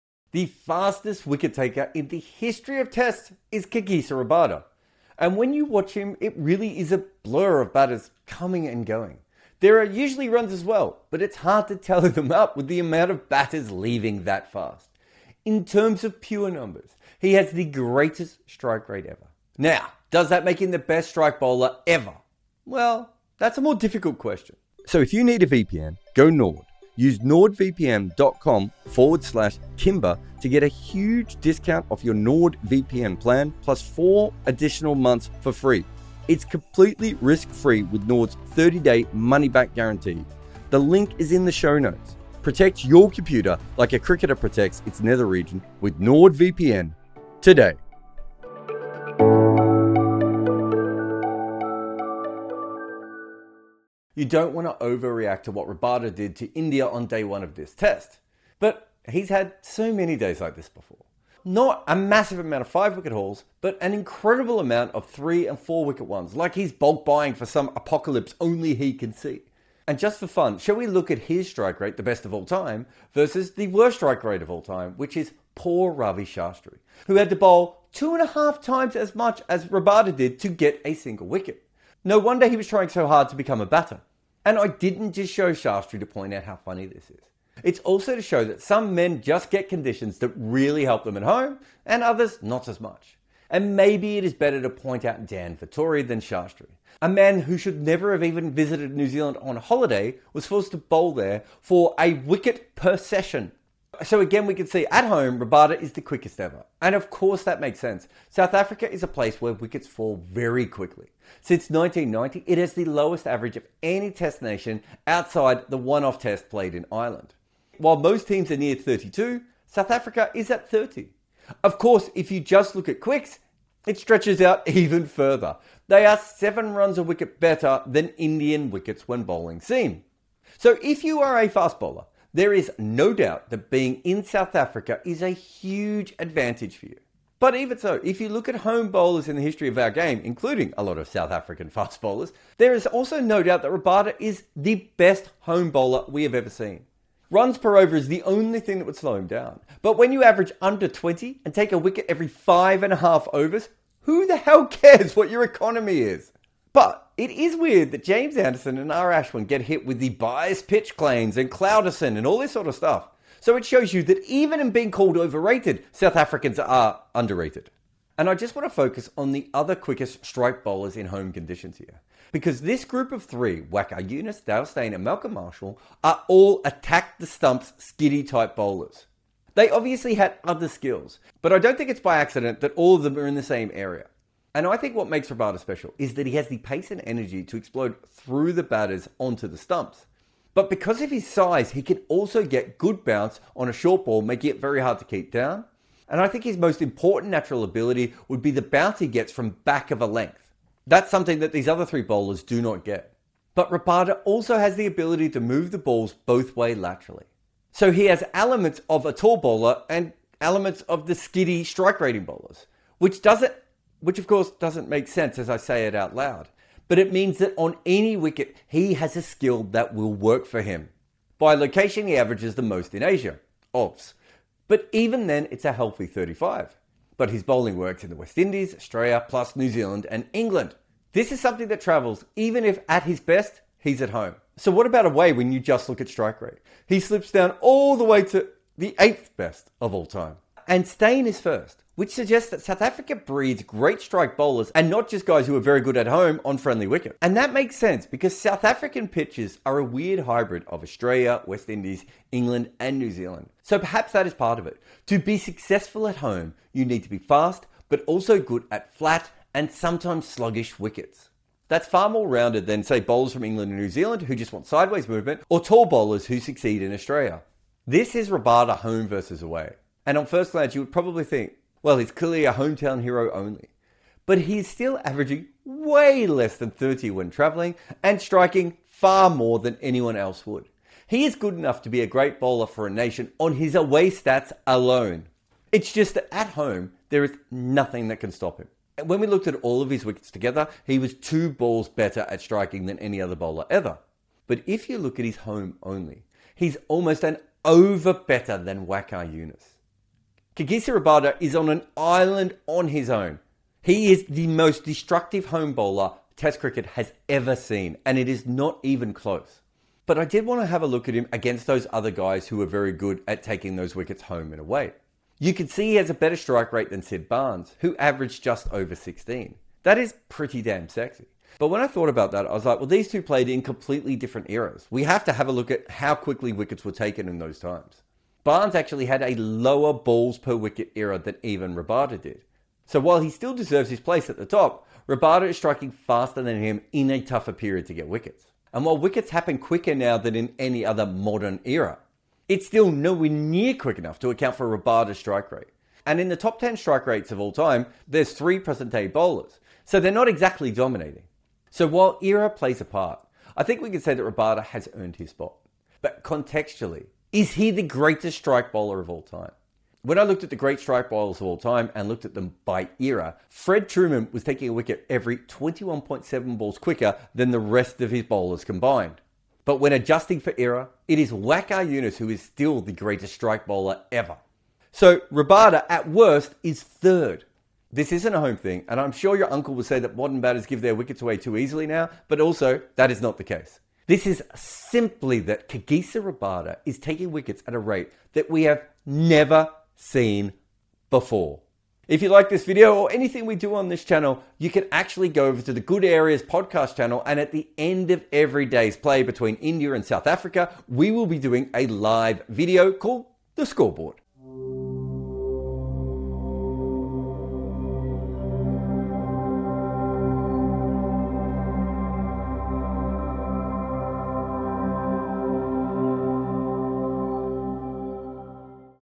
Kagiso Rabada: The fastest wicket-taker in history
🔥 Witness the sheer dominance of Kagiso Rabada, the speedster who's rewriting the history books with his mind-boggling strike rate! 🏏💨

In this cricket analysis, we delve into Rabada's unparalleled ability to send batters packing at an astonishing rate. From home ground advantage to away game prowess, we break down the numbers that make him a standout bowler.

Written by Shayan Khan and Jarrod Kimber

👍 If you're a cricket enthusiast or just love witnessing athletic greatness, this video is a must-watch!